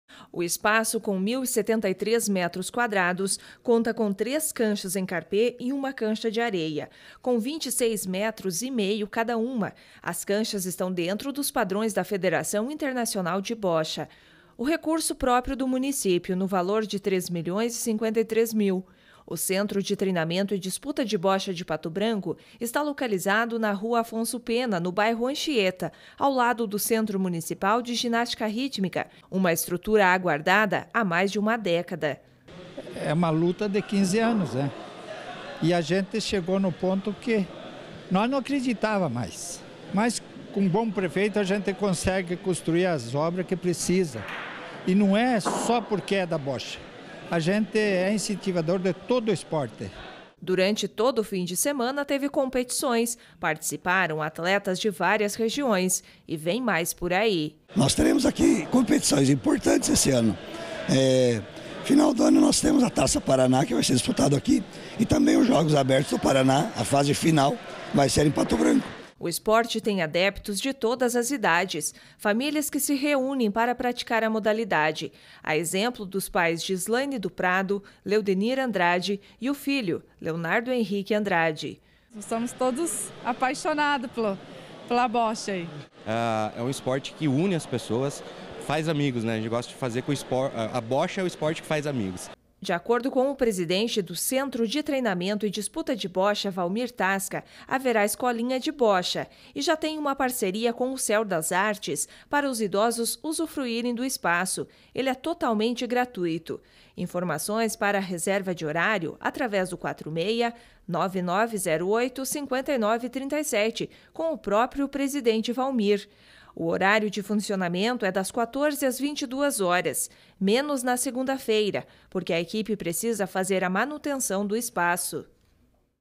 Inaugurado o Centro de Treinamento e Disputa de Bocha de Pato Branco
Esportes: O Centro de Treinamento e Disputa de Bocha de Pato Branco, foi inaugurado na sexta-feira. E teve competição durante o fim de semana.